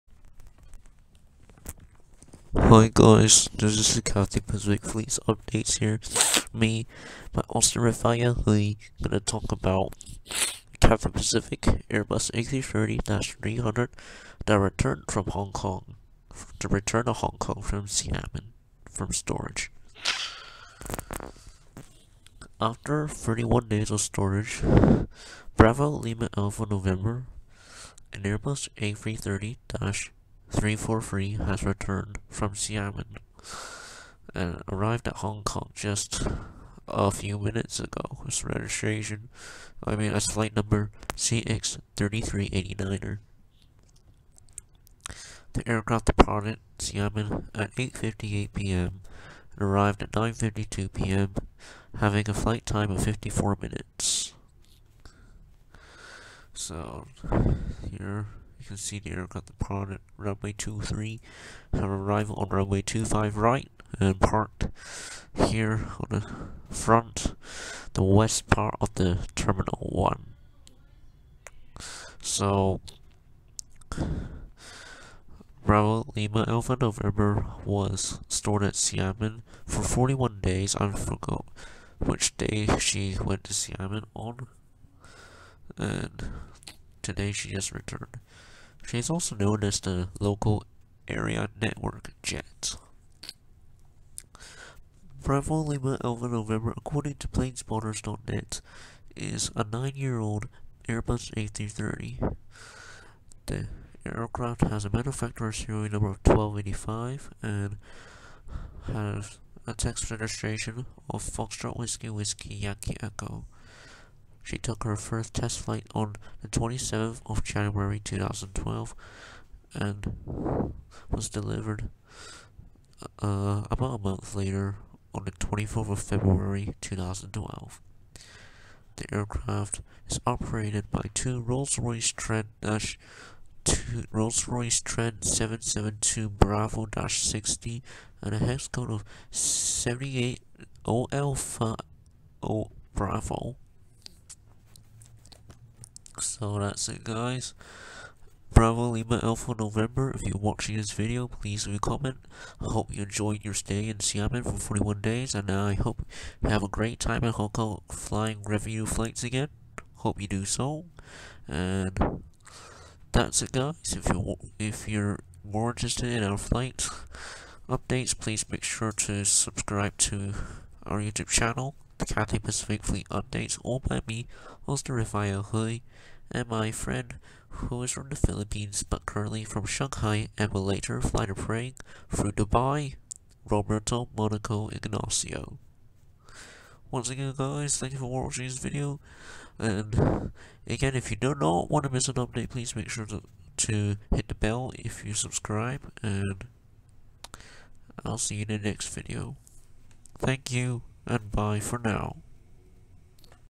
CX FLEET UPDATE: B-LAN returns to Hong Kong from storage at Xiamen - June 28th 2021
CATHAY PACIFIC UPDATE JUNE 28TH 2021: A Cathay Pacific Airbus A330-300 stored at Xiamen, has returned to Hong Kong this evening, after 41 days of storage there. She took the flight number CX3389 departing at 8:58pm local time (12:58pm UTC) and arrived at 9:52pm (1:52pm UTC). She is also nicknamed as the "Local Area Network" jet!

Aircraft details:
Registration: B-LAN
Test Registration: F-WWYE
Serial Number: 1285
Date Delivered: February 24th 2012
First Flight Date: January 27th 2012

All by Austin Raphael Hui & Roberto Monico Ignacio!

External links:
Our Cathay Pacific Fleet Spreadsheet: